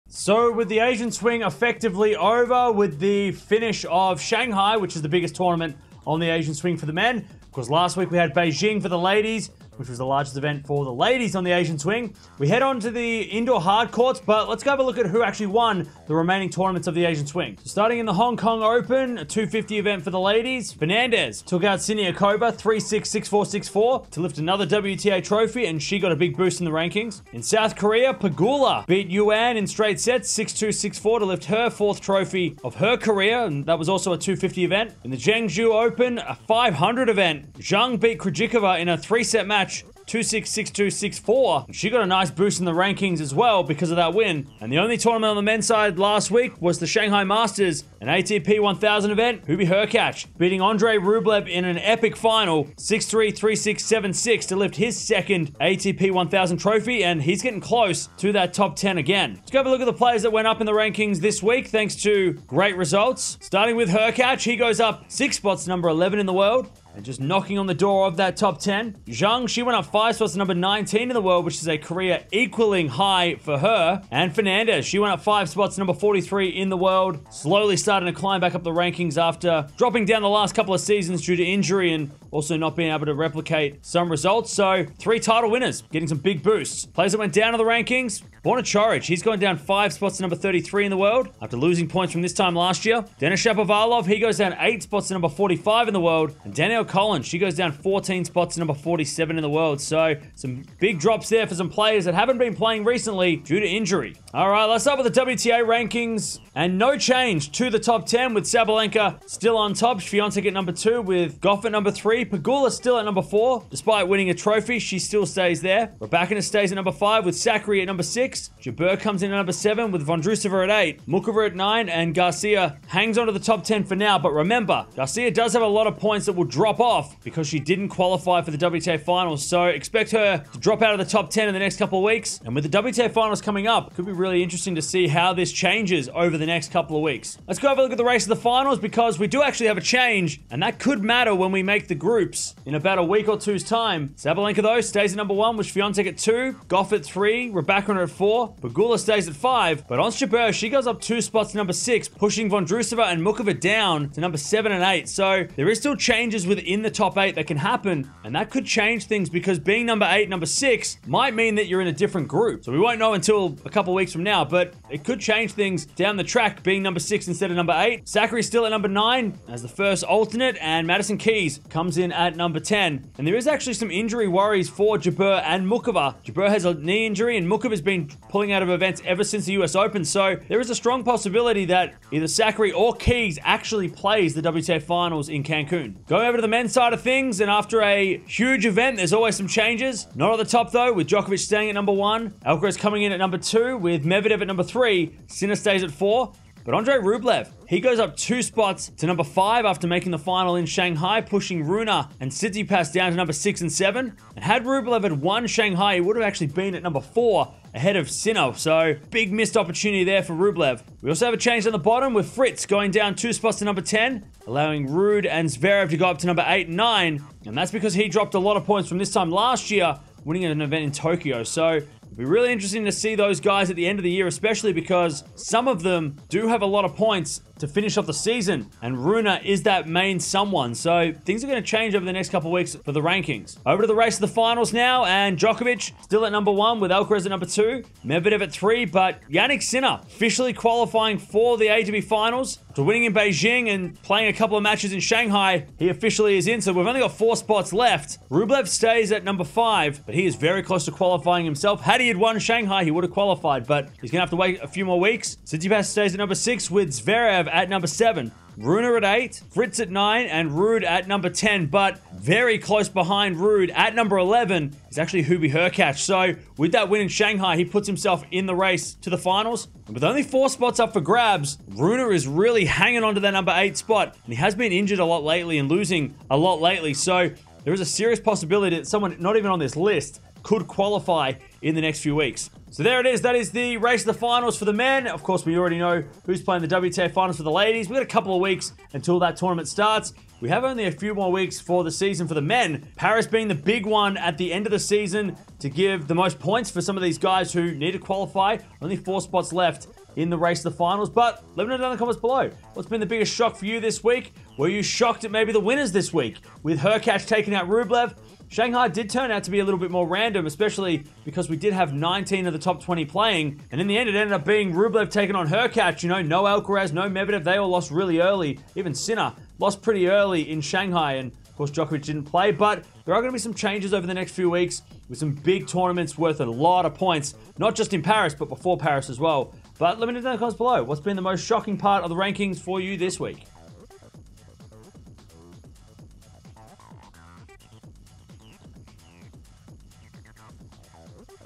Hurkacz Wins Shanghai 2023 | Tsitsipas, Rune Drop | Tennis Rankings
The Shanghai Masters are over for another year with Hurkacz defeating Rublev in the final to lift his 2nd ATP1000 trophy. Zheng, Pegula and Fernandez were among the winners on the WTA and got boosts in the rankings.

0:00 | Intro
0:17 | Past Results
0:59 | Ups & Downs
1:44 | WTA Rankings
2:17 | WTA Finals Race
3:05 | ATP Rankings
3:53 | ATP Finals Race
4:42 | Outro